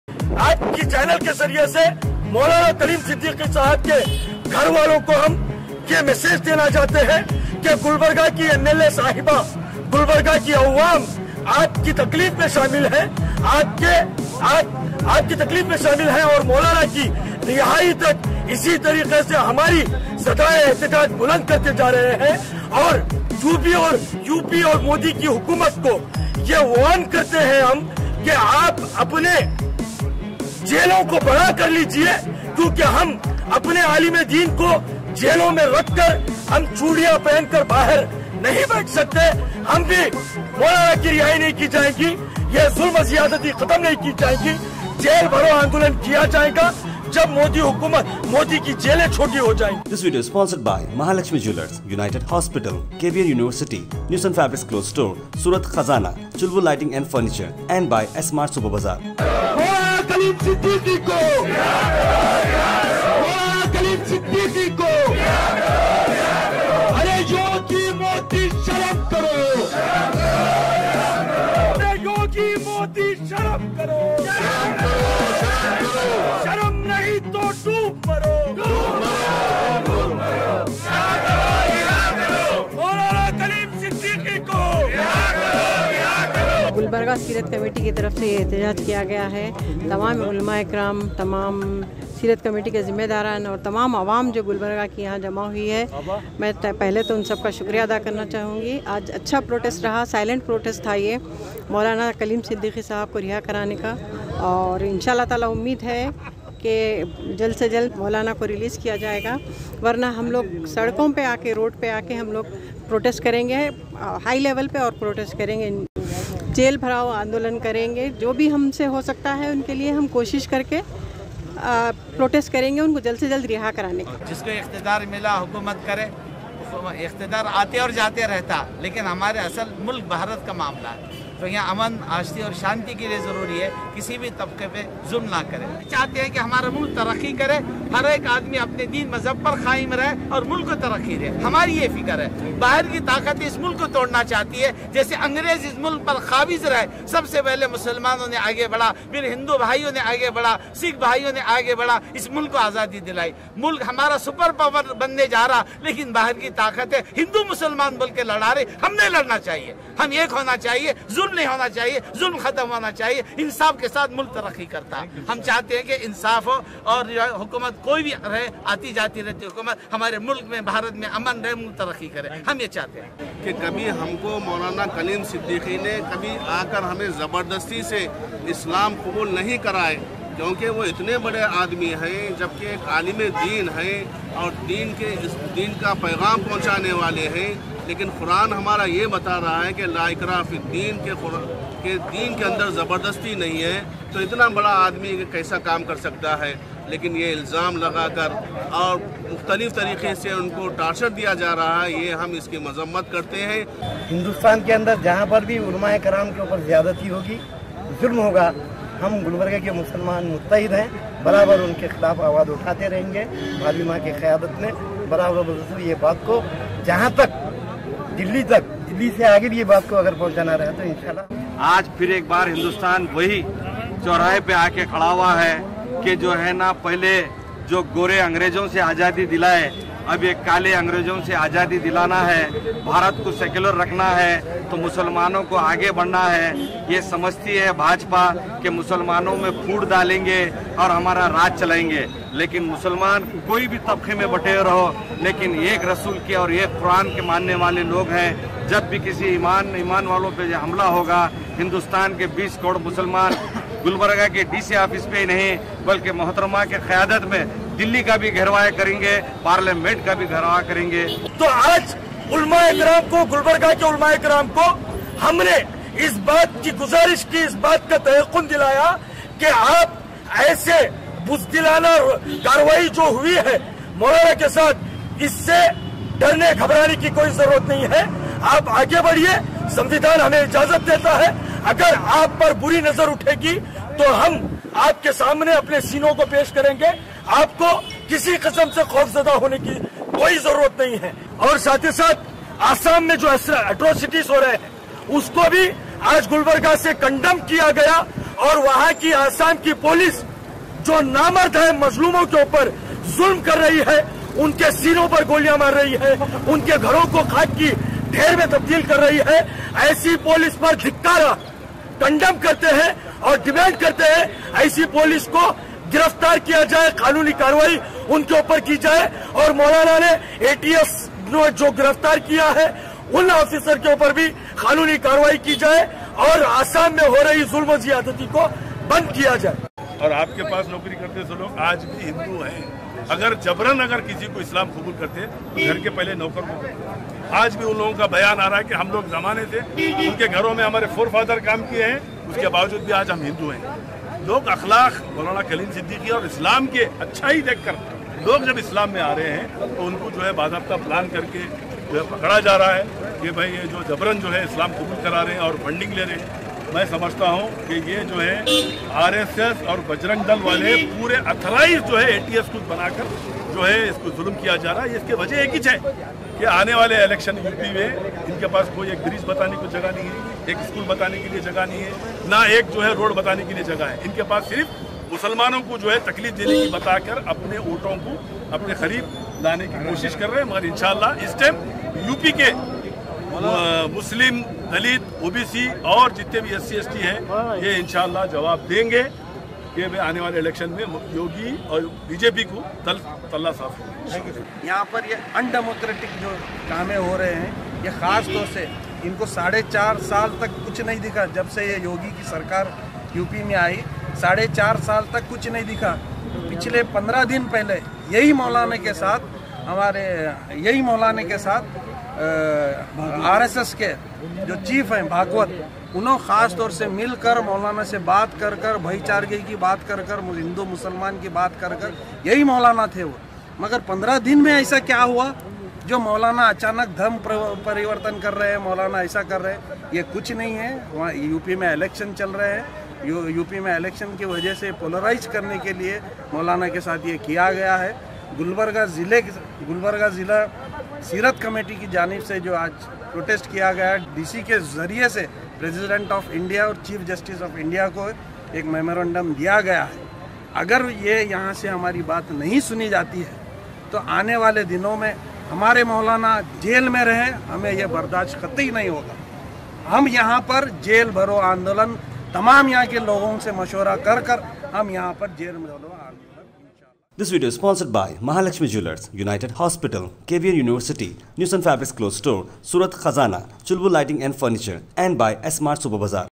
MARKAZI SEERATH COMMITTEE ORGANISED PROTEST AT DC OFFIC
MARKAZI SEERATH COMMITTEE ORGANISED PROTEST AT DC OFFICE
DEMANDS IMMEDIATE RELEASE MAULANA KALEEM SIDDIQUI SAHAB